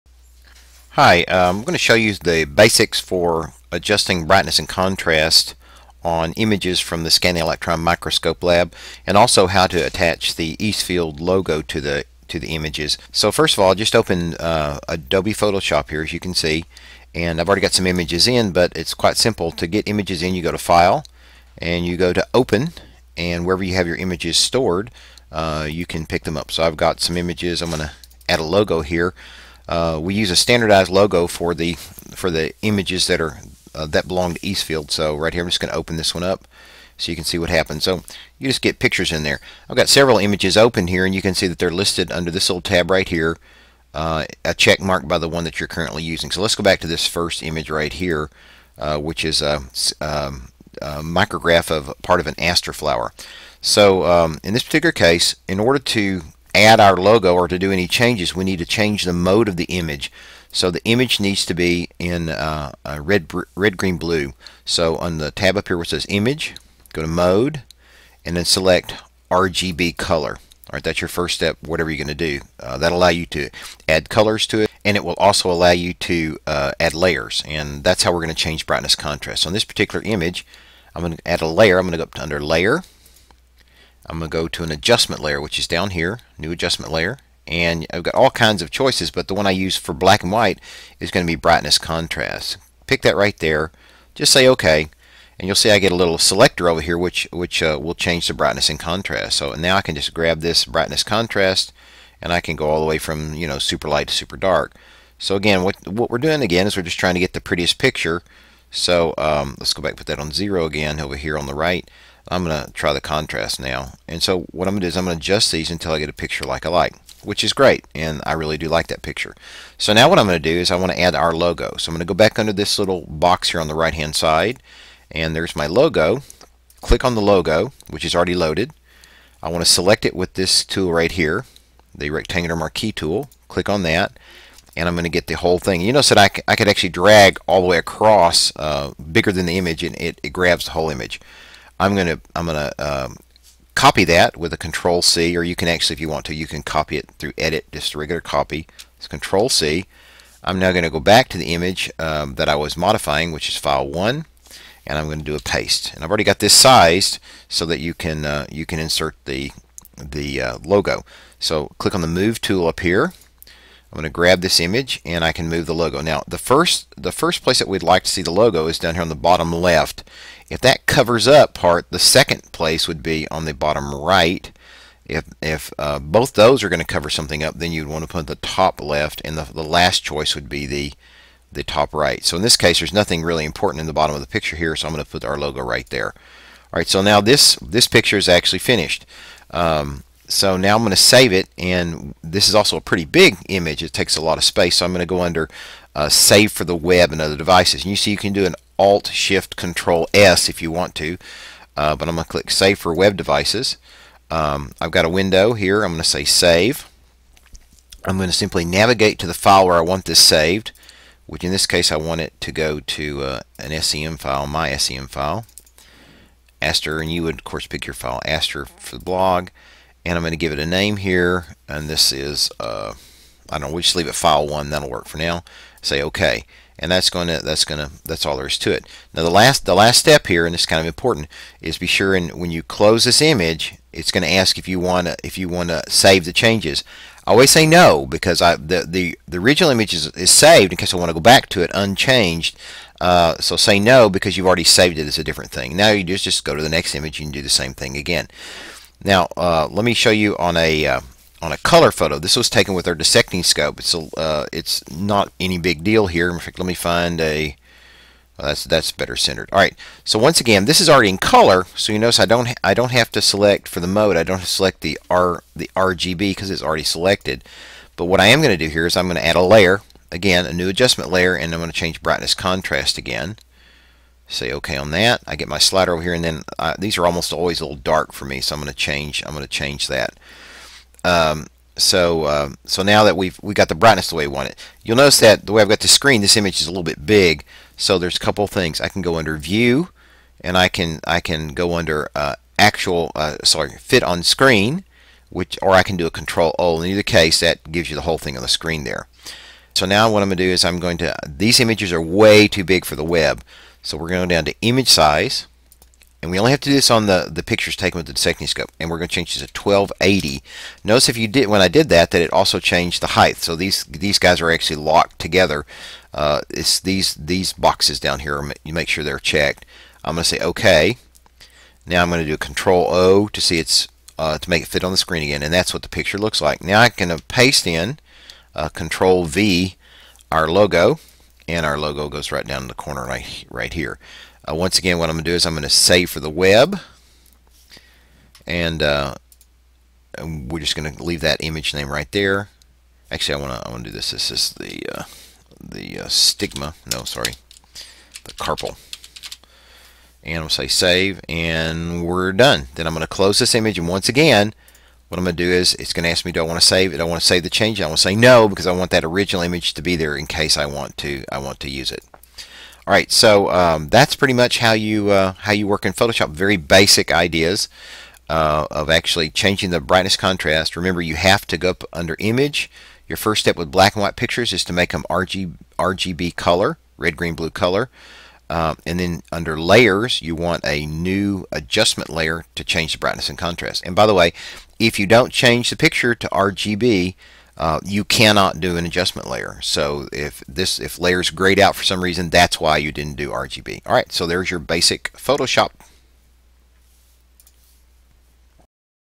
Using Photoshop on SEM Images
This video shows how to use photoshop to change the brightness and contrast of the images taken in the SEM Lab.  It also shows how to resize images and how to save them for the web.